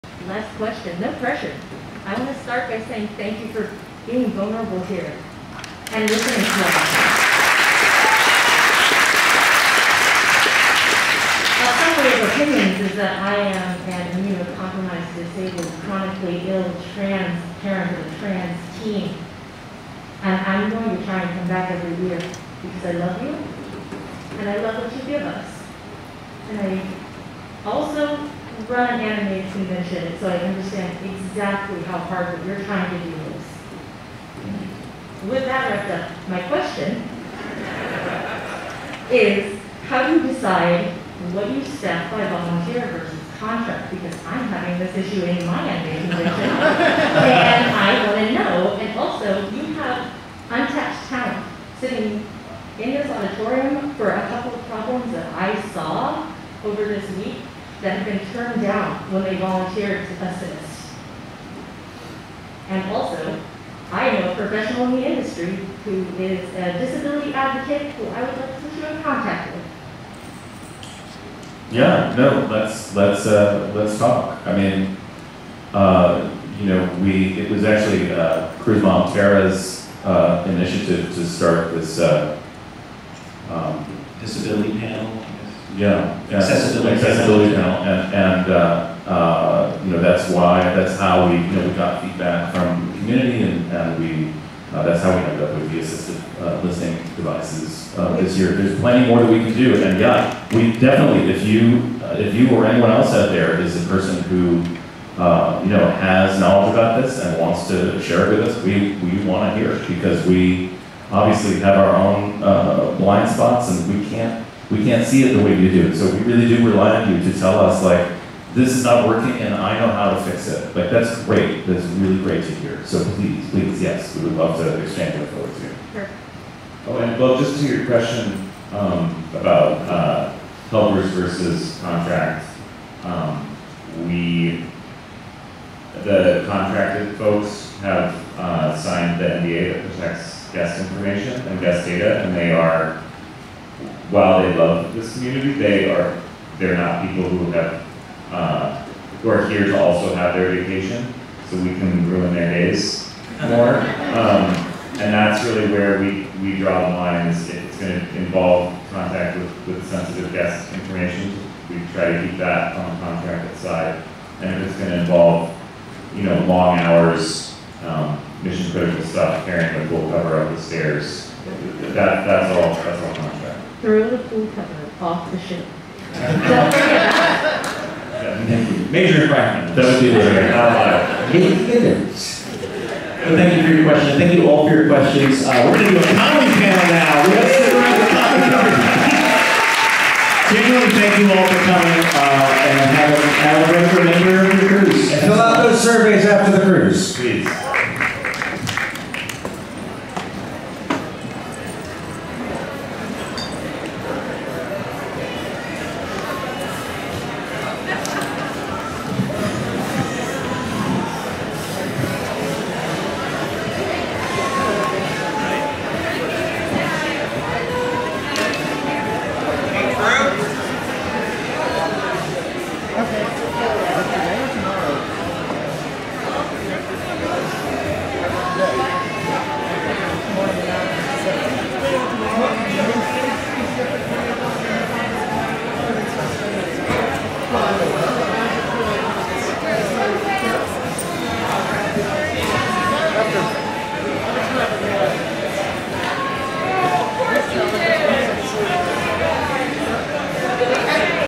How do you decide what to staff by volunteer vs. contract? — Management Q&A on JoCo Cruise 2023
Q: Thank you for being vulnerable! I’m a chronically ill disabled trans person who runs an anime convention, I understand exactly how hard this is. How do you decide what to staff by volunteer vs. contract?

All the Q&A I've recorded from JoCo Cruise and JoCo Cruise-adjacent performers and performances can be found